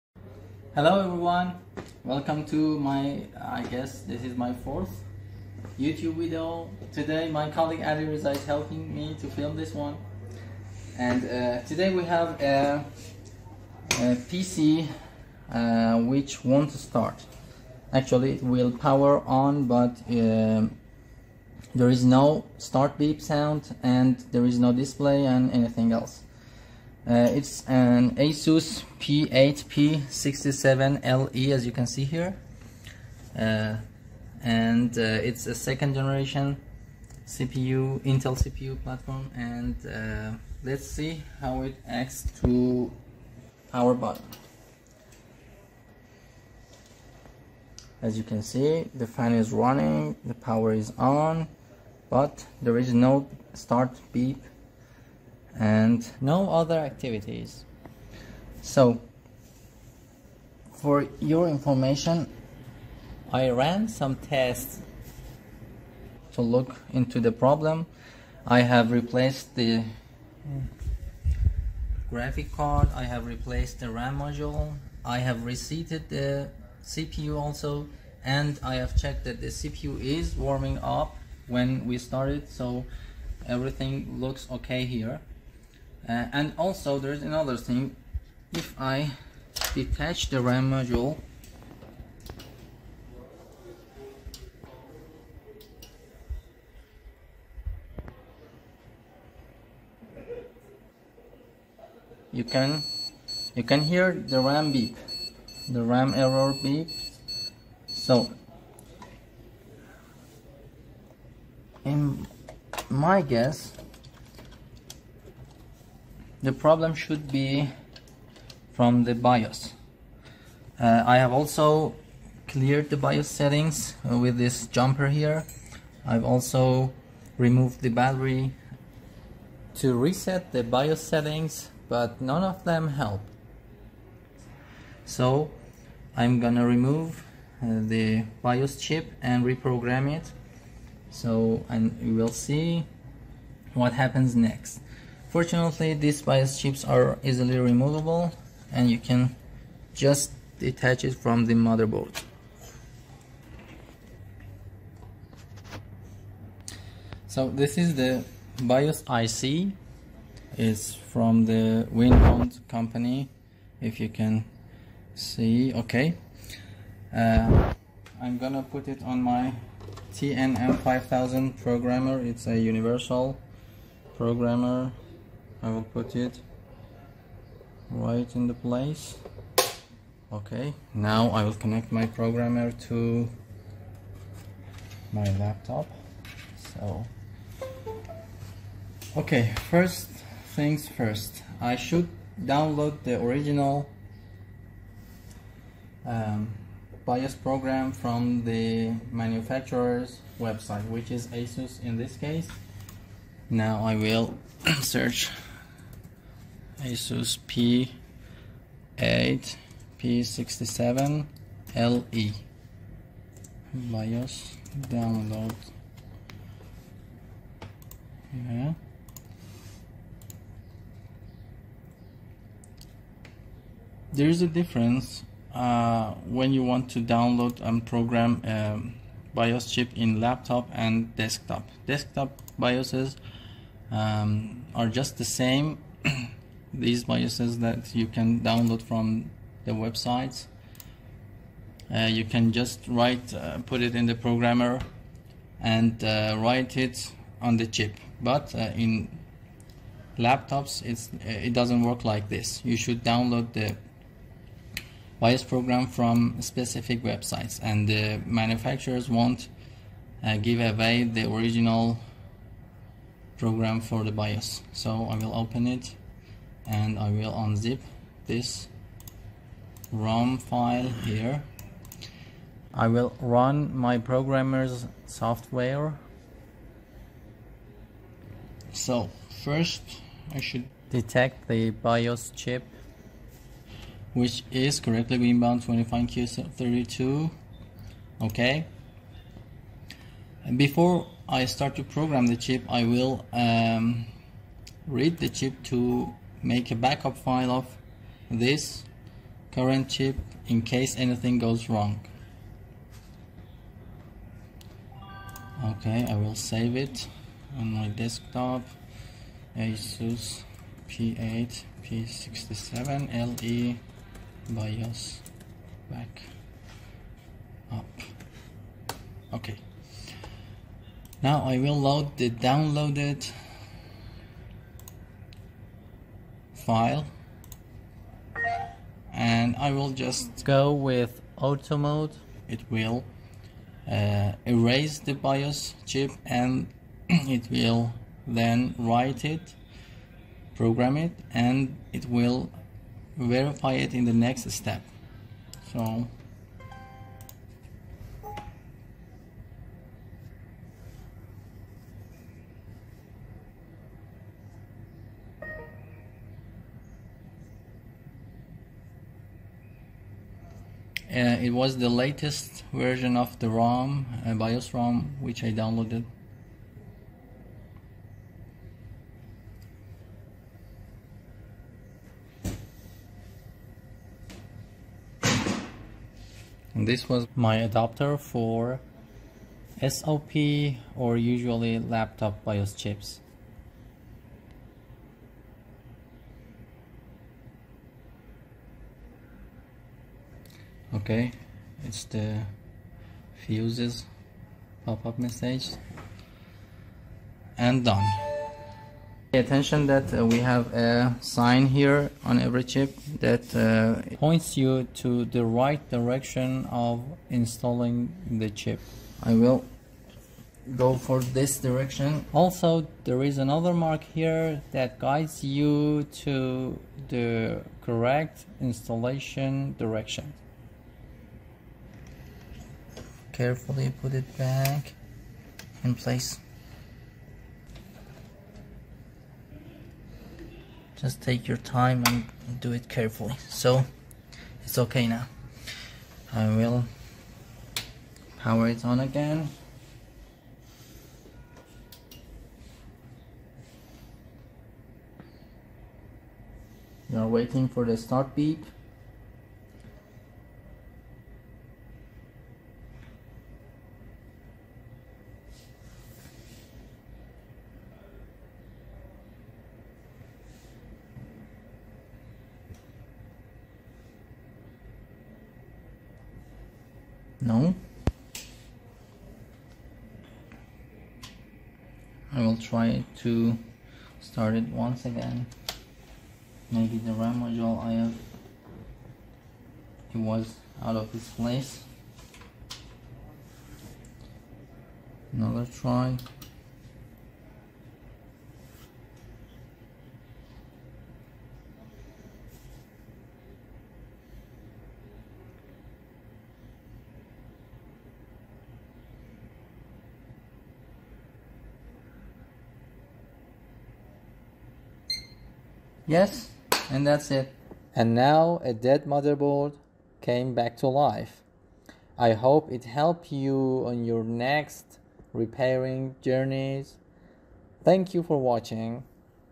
ASUS Motherboard Starts with no Display and Beep - Corrupt BIOS
Here we have an ASUS P8P67-LE Motherboard (2nd & 3rd gen intel CPU compatible) which starts with no Display and no Start Beep (Stuck at running CPU and PSU fans). I've tested the RAM, Graphics Card and CPU and they were OK. BIOS hard reset also didn't help it.
BIOS chip reprogramming fixed the issue.